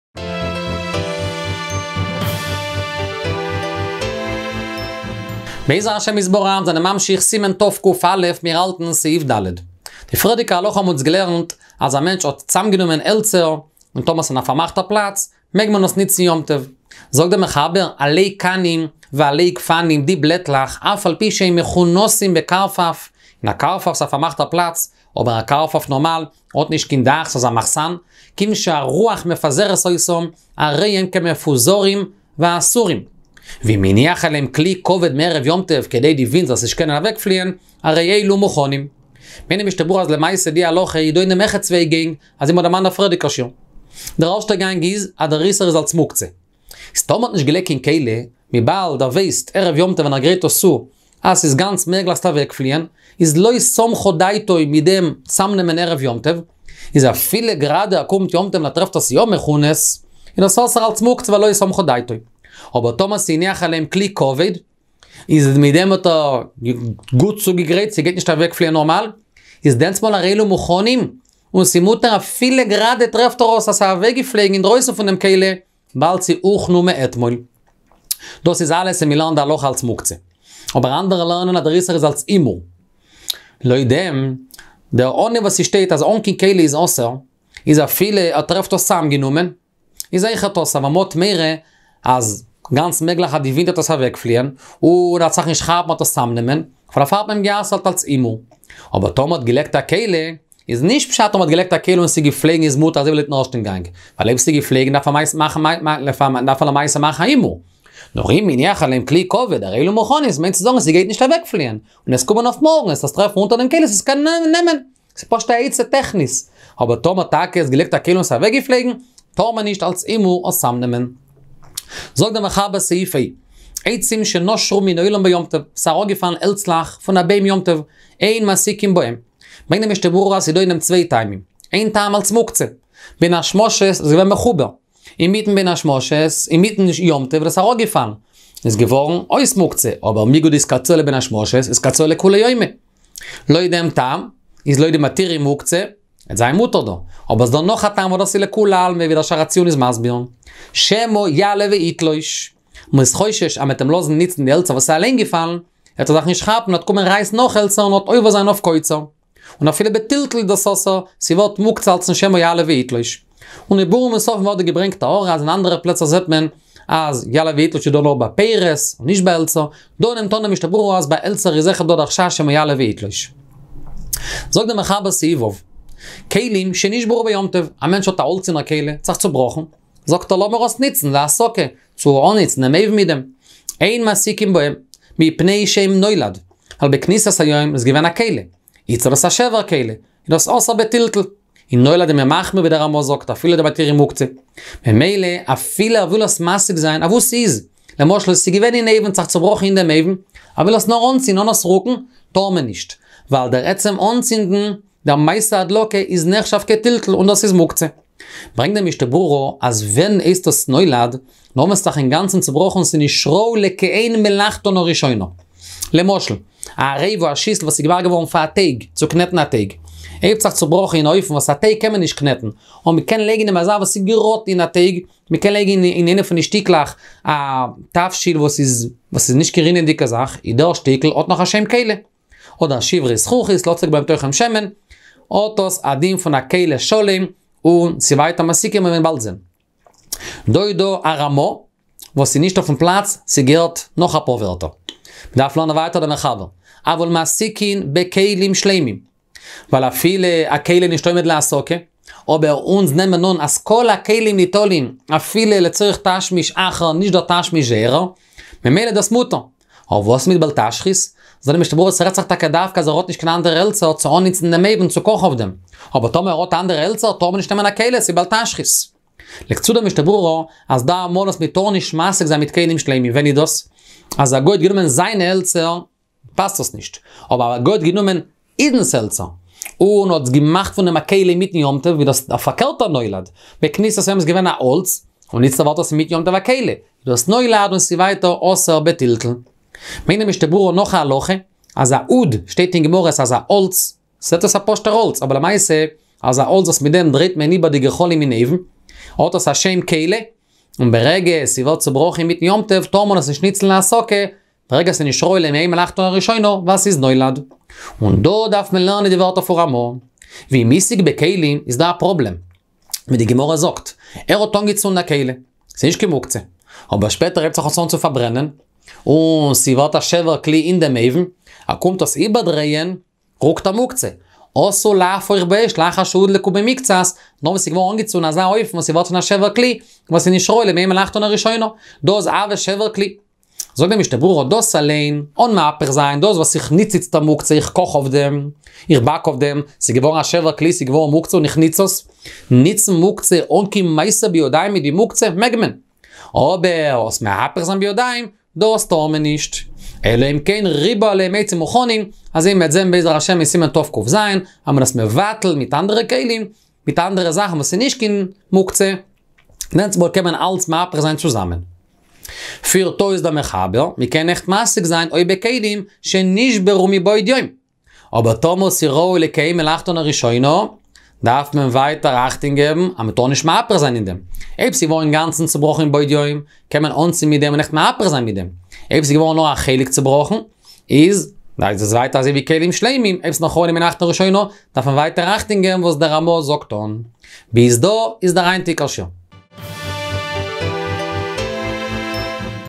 האם מותר להשתמש במוקצה וליהנות הימנו?- סימן תק"א- סעיף ד'- סעיף ו'
שיעורי ה'דף היומי בהלכה' באידיש בספר משנ"ב מבית "דרשו" עם הרב אפרים סגל שליט"א, רוכשים ידיעות בהלכה.
סימן תק"א- סעיף ד'- סעיף ו' - שיעור 562